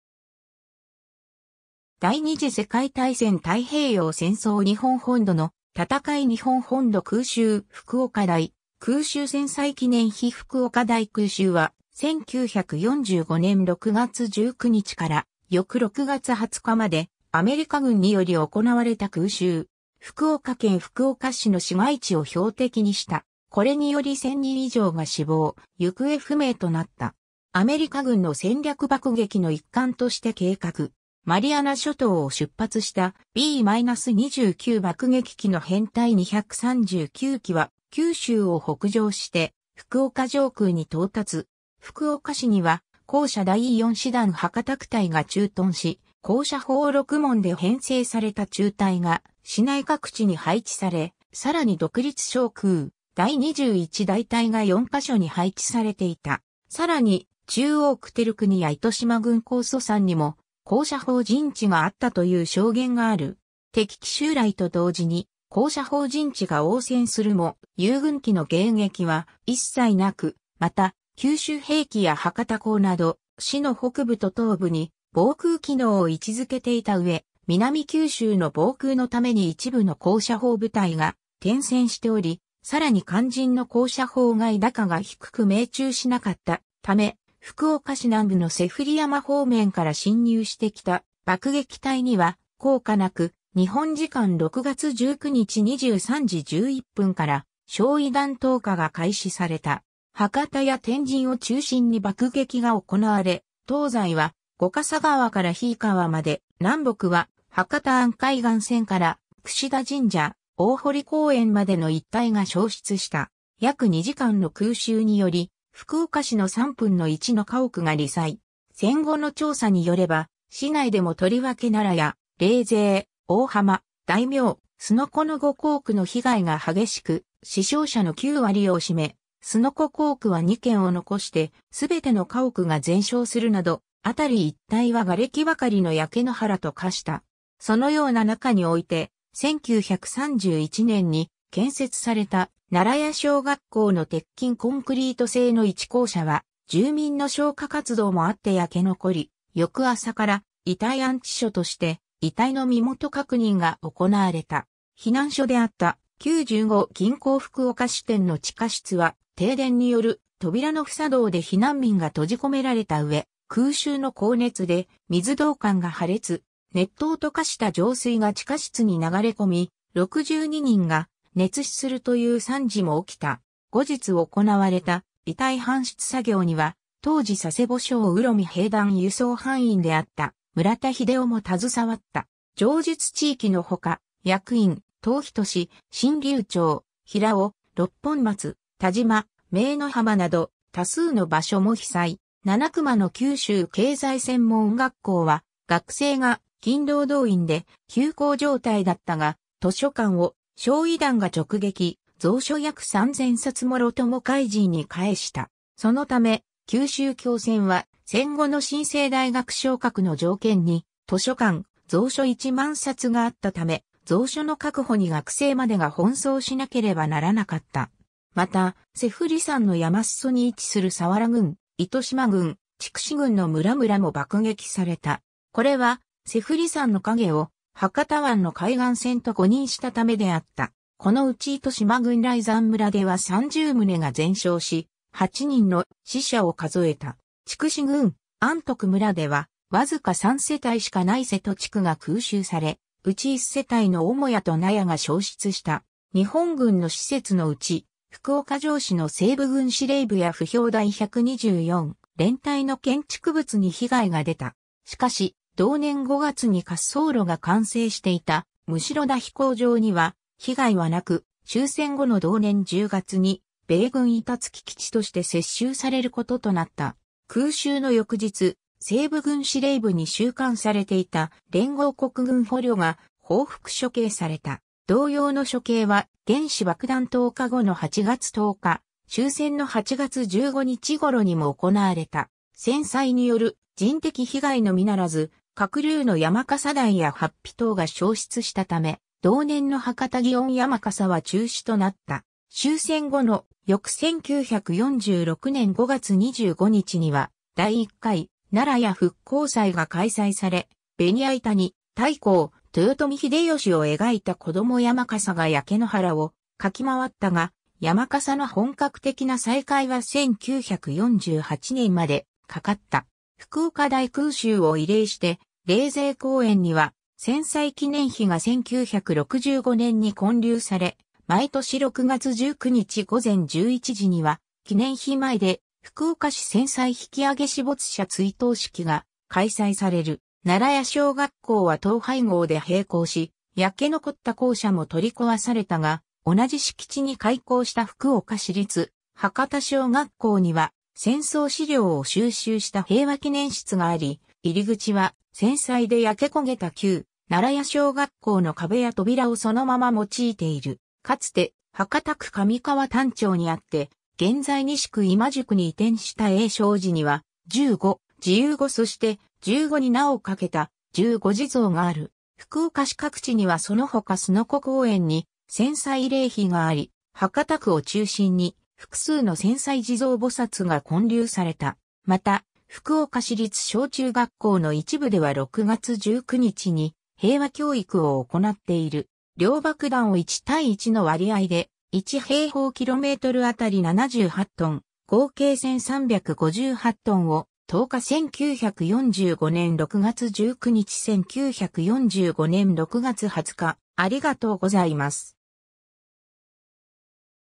福岡大空襲
第二次世界大戦 」 太平洋戦争 」 日本本土の戦い 」 日本本土空襲 」 福岡大空襲 戦災記念碑 福岡大空襲（ふくおかだいくうしゅう）は、1945年6月19日から翌6月20日までアメリカ軍により行われた空襲。
福岡県福岡市の市街地を標的にした。
これにより1,000人以上が死亡・行方不明となった。
 アメリカ軍の戦略爆撃の一環として計画。
マリアナ諸島を出発したB-29爆撃機の編隊239機は九州を北上して福岡上空に到達。
福岡市には高射第4師団博多区隊が駐屯（本部：東平尾）し、高射砲6門で編成された中隊が市内各地に配置され、さらに独立照空第21大隊（本部：筑紫郡春日村、現春日市）が4ヵ所に配置されていた。
さらに、中央区輝国や糸島郡高祖山にも高射砲陣地があったという証言がある。
敵機襲来と同時に高射砲陣地が応戦するも、友軍機の迎撃は一切無く、また九州兵器（現・渡辺鉄工所本社工場）や博多港など市の北部と東部に防空機能を位置づけていた上、南九州の防空のために一部の高射砲部隊が転戦しており、さらに肝心の高射砲が射高が低く命中しなかったため、福岡市南部の脊振山方面から進入してきた爆撃隊には効果なく、日本時間6月19日23時11分から焼夷弾投下が開始された。
 博多や天神を中心に爆撃が行われ、東西は御笠川から樋井川まで、南北は博多湾海岸線から櫛田神社・大濠公園までの一帯が焼失した。
約2時間の空襲により福岡市の3分の1の家屋が罹災。
戦後の調査によれば、市内でもとりわけ奈良屋・冷泉・大浜・大名・簀子の5校区の被害が激しく、死傷者の9割を占め、簀子校区は2軒を残して全ての家屋が全焼するなど、あたり一帯は瓦礫ばかりの焼け野原と化した。
そのような中において、1931年に建設された奈良屋小学校（現在の博多小学校の立地）の鉄筋コンクリート製の一校舎は住民の消火活動もあって焼け残り、翌朝から遺体安置所として遺体の身元確認が行なわれた。
 避難所であった旧十五銀行福岡支店（現在の博多座の立地）の地下室は、停電による扉の不作動で避難民が閉じ込められたうえ、空襲の高熱で水道管が破裂。
熱湯と化した上水が地下室に流れ込み、62人が熱死するという惨事も起きた。
後日行われた遺体搬出作業には、当時佐世保相浦海兵団輸送班員であった村田英雄も携わった。
 上述地域のほか、薬院・当仁・新柳町（現在の清川）・平尾・六本松・田島・姪浜など多数の場所も被災。
七隈の九州経済専門学校（現在の福岡大学）は学生が勤労動員で休校状態だったが、図書館を焼夷弾が直撃、蔵書約3,000冊もろとも灰燼に帰した。
そのため、九州経専は戦後の新制大学昇格の条件に図書館蔵書1万冊があったため、蔵書の確保に学生までが奔走しなければならなかった。
また脊振山の山裾に位置する早良郡・糸島郡・筑紫郡の村々も爆撃された。
これは脊振山の影を博多湾の海岸線と誤認したためであった。
このうち糸島郡雷山村（現在の糸島市雷山地区）では30棟が全焼し、8人の死者を数えた。
筑紫郡安徳村（現在の那珂川市安徳）では、わずか3世帯しかない瀬戸地区が空襲され、うち1世帯の母屋と納屋が焼失した（集落東側の採石場を標的にしたと考えられている）。
 日本軍の施設のうち、福岡城址の西部軍司令部や歩兵第124連隊の建築物に被害が出た。
しかし同年5月に滑走路が完成していた席田飛行場（現在の福岡空港）には被害はなく、終戦後の同年10月に米軍板付基地として接収されることとなった。
 空襲の翌日、西部軍司令部に収監されていた連合国軍捕虜が報復処刑された。
同様の処刑は原子爆弾投下後の8月10日、終戦の8月15日頃にも行われた。
 戦災による人的被害のみならず各流の山笠台や法被等が焼失したため、同年の博多祇園山笠は中止となった。
終戦後の翌1946年5月25日には「第一回奈良屋復興祭」が開催され、ベニヤ板に太閤豊臣秀吉を描いた子供山笠が焼け野原を舁き回ったが、山笠の本格的な再開は1948年までかかった。
 福岡大空襲を慰霊して、冷泉公園には戦災記念碑（作：伊藤研之）が1965年に建立され、毎年6月19日午前11時には記念碑前で「福岡市戦災引揚死没者追悼式」が開催される。
奈良屋小学校は統廃合で閉校し、焼け残った校舎も取り壊されたが、同じ敷地に開校した福岡市立博多小学校には戦争資料を収集した平和記念室があり、入口は戦災で焼け焦げた旧奈良屋小学校の壁や扉をそのまま用いている。
かつて博多区上川端町にあって現在西区今宿に移転した栄昌寺に...